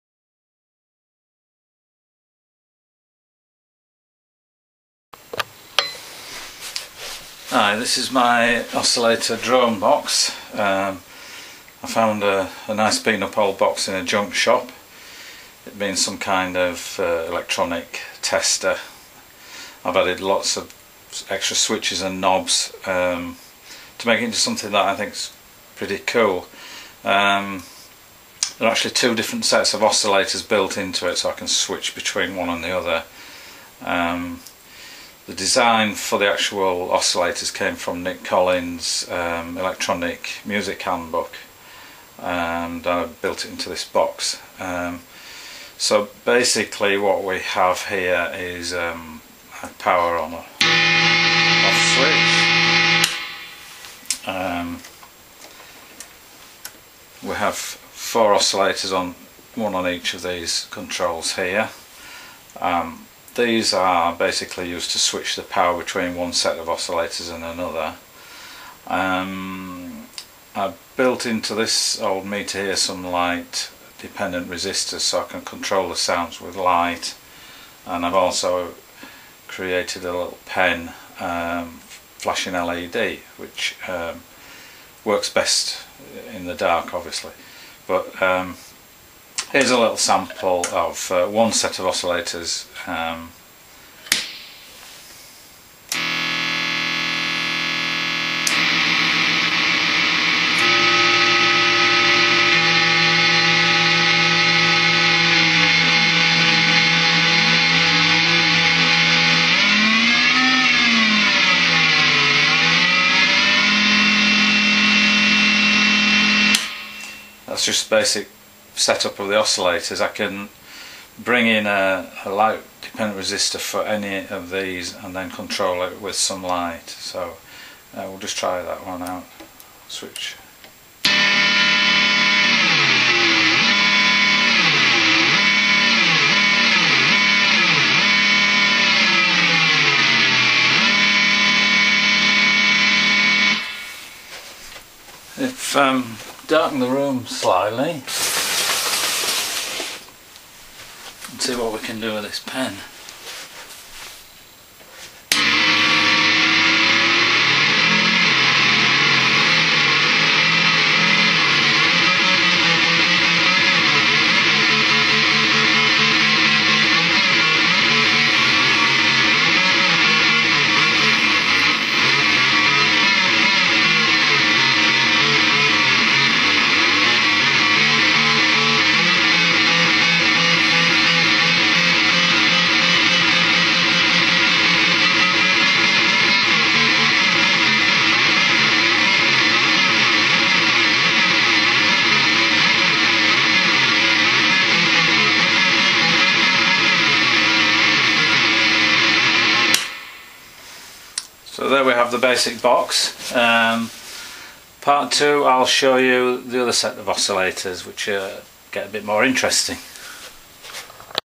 The Pulsillator Part 1
A hand made oscillator/drone box inspired by Nic Collins Handmade Electronic Music book. I call it The Pulsillator. The box is an old electrical tester. I fitted one set of hex inverters and one set of cascaded nand inverters, each providing four seperate oscillators controlled by the four pots with switchable light dependent resistors. The light pen can be hooked to one of the oscillator outputs to provide a flashing light to use on the LDRs. Sounds crap on this vid but put through some FX and a decent amp it's wicked.